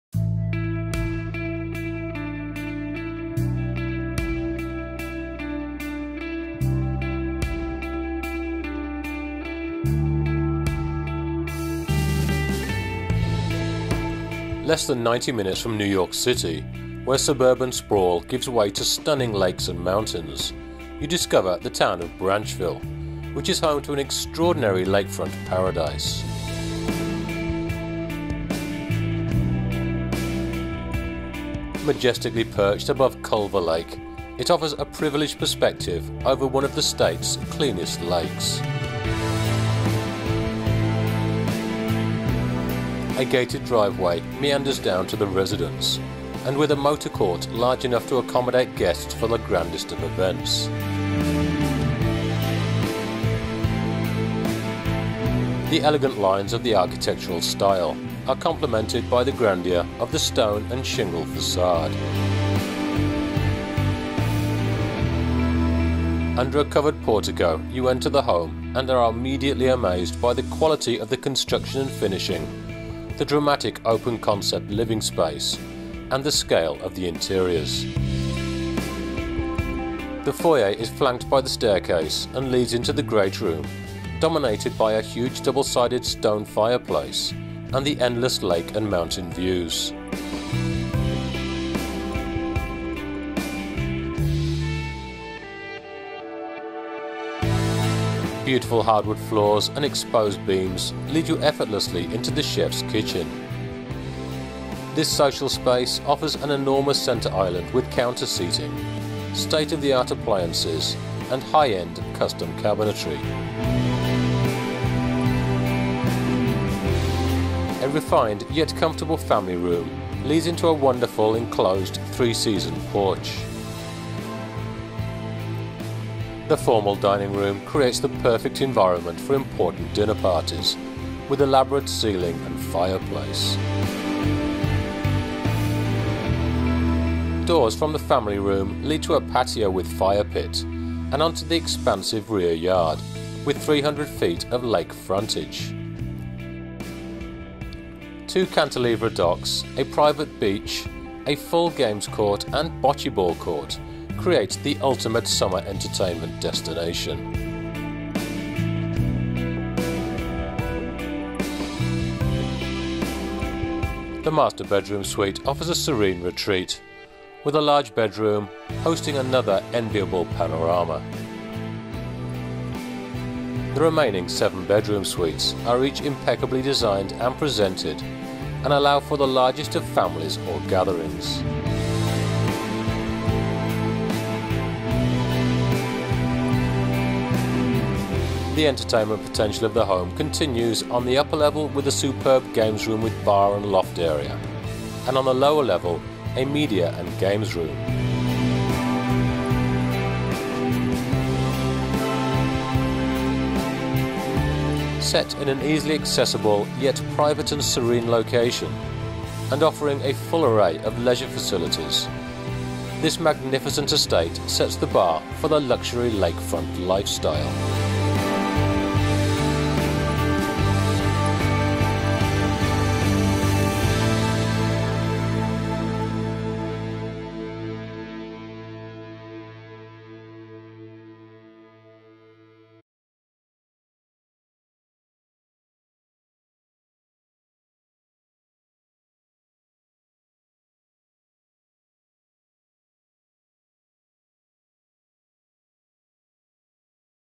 Majestic Paradise in Frankford Township, New Jersey | Sotheby's International Realty
Presented by Prominent Properties Sotheby's International Realty

This majestic, custom built lakefront paradise sits on over 8 acres of breath taking property with 300ft lake frontage on the highly desirable Culver Lake. Upon entry, you’ll be greeted by an open concept living space and soaring ceilings offering modern amenities and endless luxuries. The first floor master suite has a full bathroom and amazing lake and mountain views. You’ll also find a great room with plumbing for a bar and dual fire place, over sized dining room, family room and state of the art gourmet kitchen with an enormous center island for casual dining as well as an enclosed 3 season porch. There’s a total of 8 bedrooms , including 2 junior suites, and 8 bathrooms. 2nd floor Game room is equipped with a bar and loft area, just perfect for entertaining and watching the sunset. Nearly every room has a spectacular view of the lake. With Custom wood work, hand scraped engineered hardwood flooring in all bedrooms, as well as porcelain wood tile, radiant heat, alarm and camera system, Crestron home system though. And to enjoy the outdoors you find 2 cantilever docks, private beach area, full basketball/tennis/volley ball and Bocce courts. 3 car garage plus additional garage to store the ATV’s canoe and other lake.

Property ID: PPTXE6